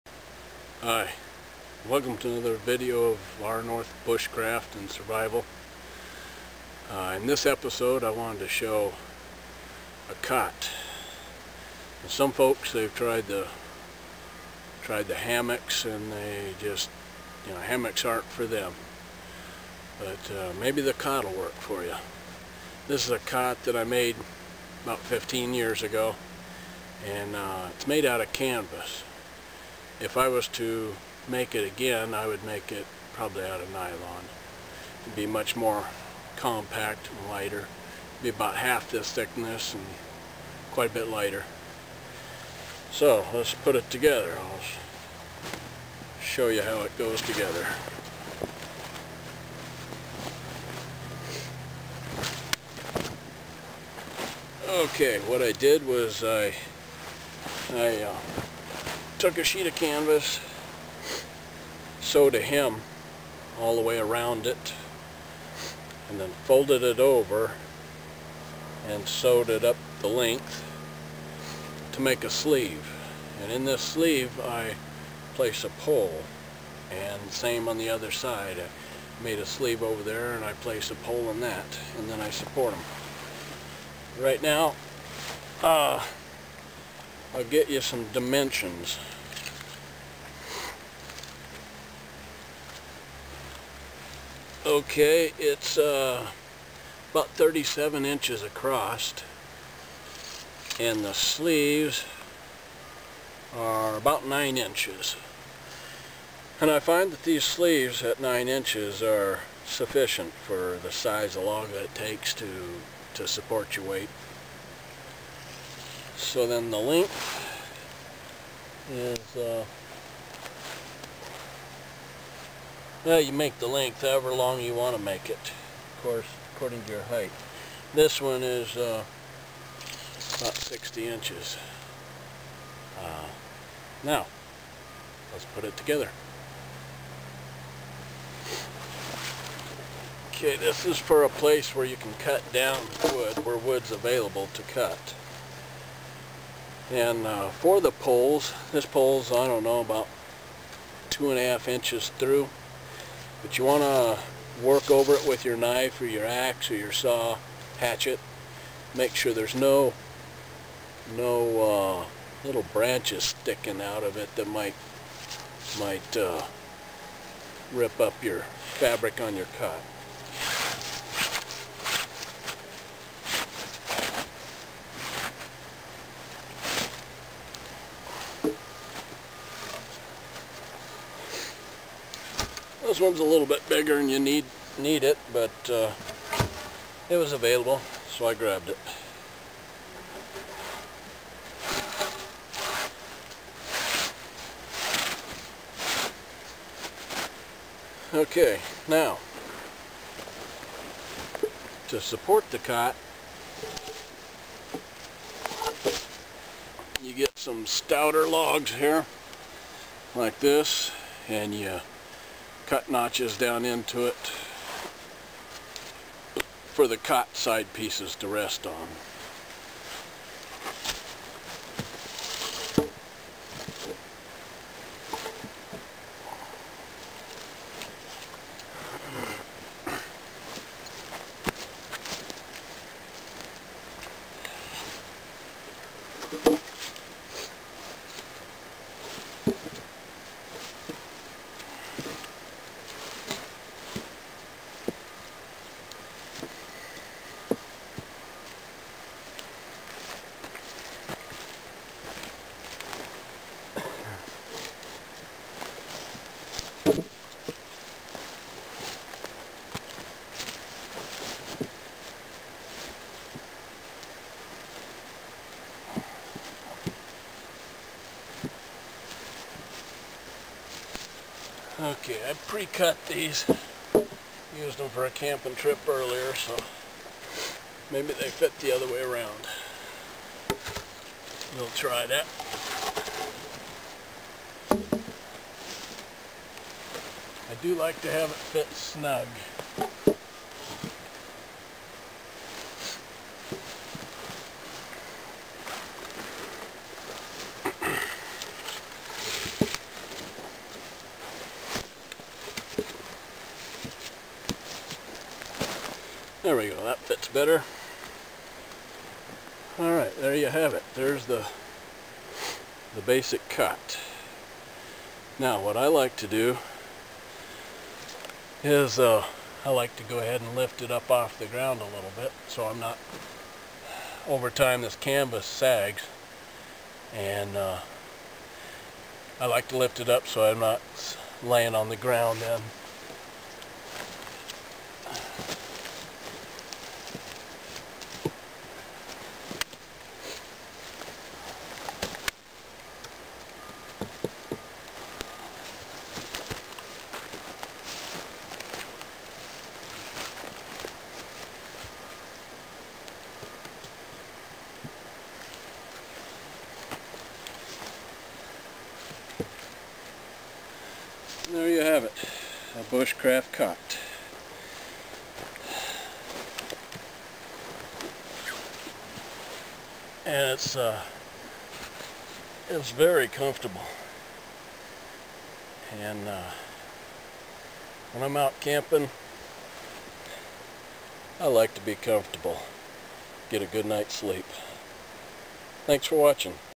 A Simple Homemade Camping Cot
I demonstrate how to make an extremely compact light weight camping cot.   here you will find the dimensions as well as types of material recommended.   Also demonstrated is how to set up and use the cot.   Hope you enjoy and find it useful.